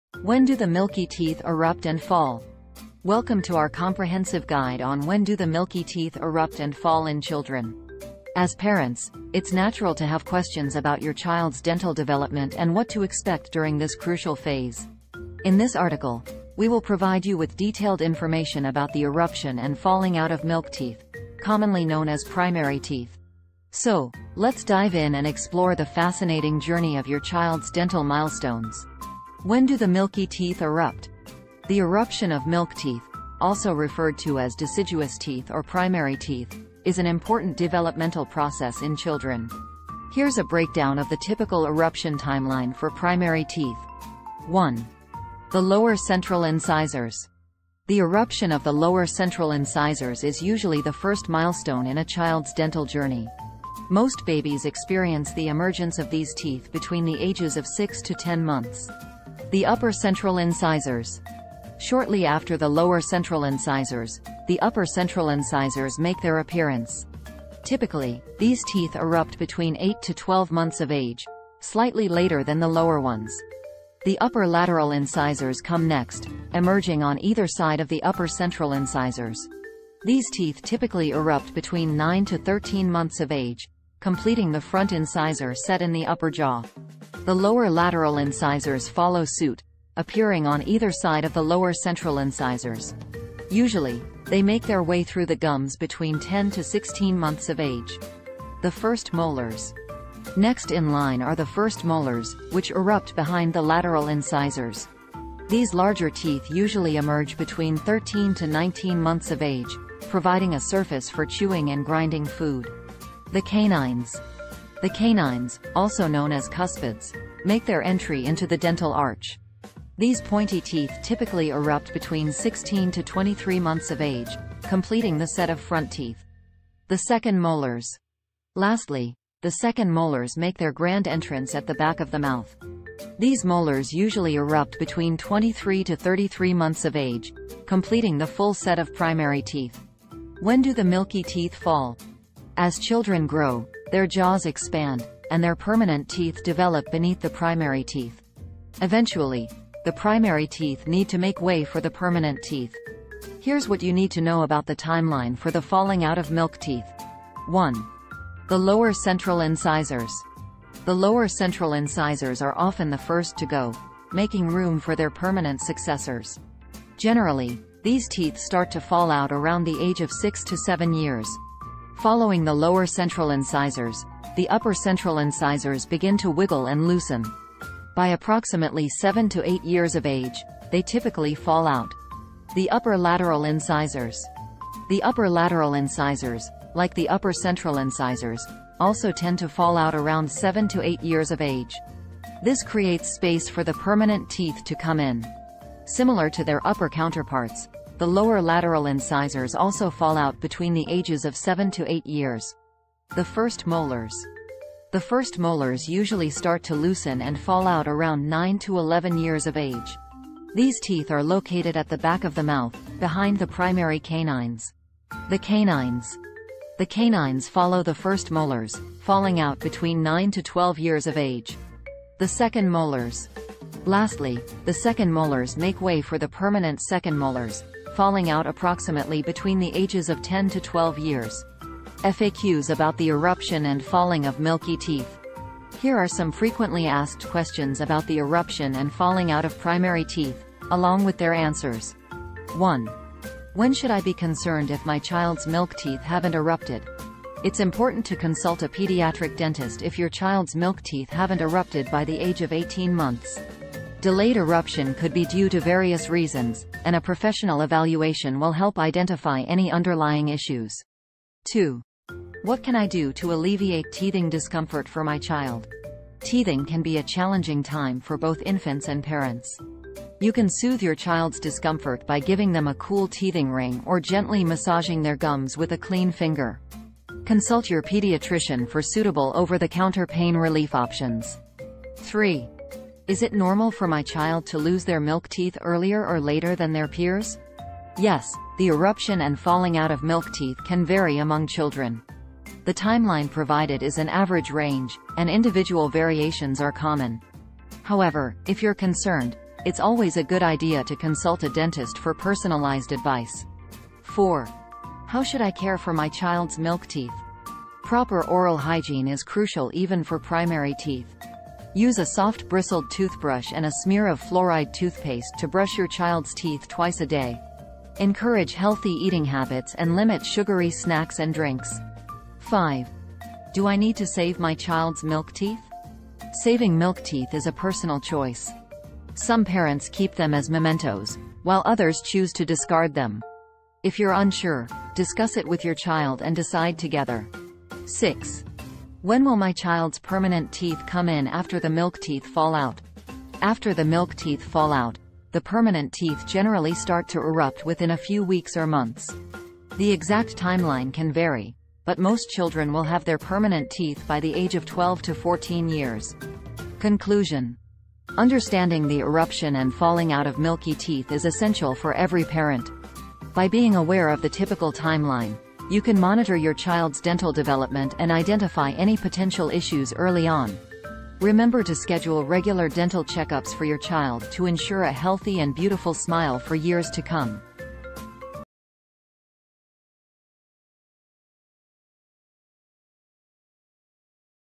"Unlocking the Secrets of Milky Teeth: Must-Watch Video!"
In this captivating video, we delve deep into the world of Milky Teeth and reveal all the essential information parents need to know. From their significance to potential health risks, we leave no stone unturned. Join us as we unlock the secrets surrounding Milky Teeth and provide valuable insights to help you understand and navigate this crucial phase of your child's dental development.